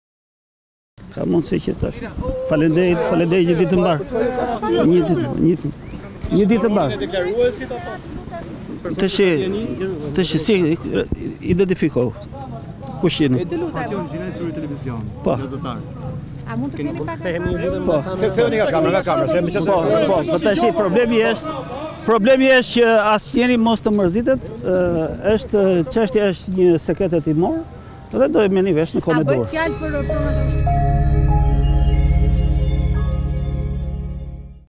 Report TV -Akuzat për privatizimin e pronave të ushtrisë në Sarandë, Kollçaku: Sekret hetimor!
Deputeti Bardhyl Kollcaku nuk ka dhënë detaje për akuzat lidhur me privatizimin e pronave të ushtrisë në Sarandë, për të cilat u paraqit sot në SPAK.

“Është sekret hetimor, do të informoheni në momentin e duhur”, tha Kollçaku pasi doli nga Prokuroria e Posaçme.